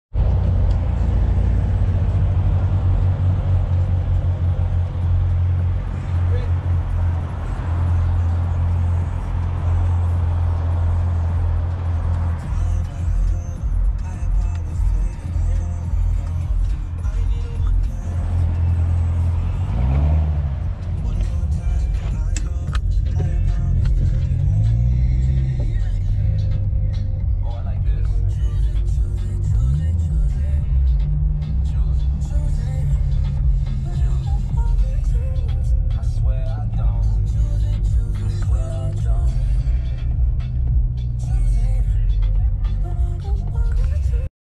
Black corvette C7 Z06
Two all black C7 corvette about to race